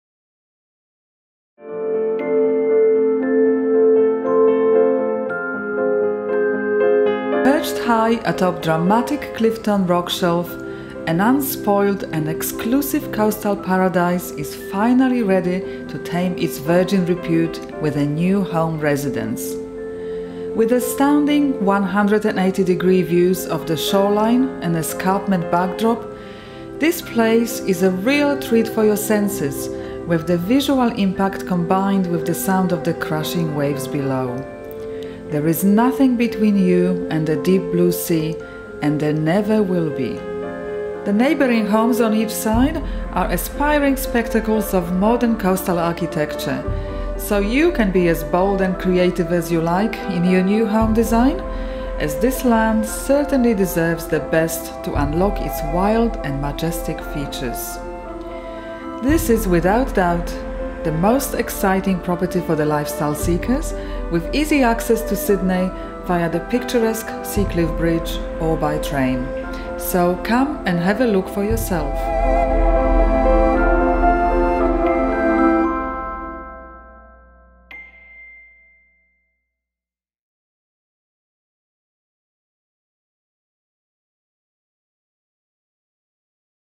339 Lawrence Hargrave Drive, Clifton
Views to Infinity...
Vacant land on cliff top position to build your dream home.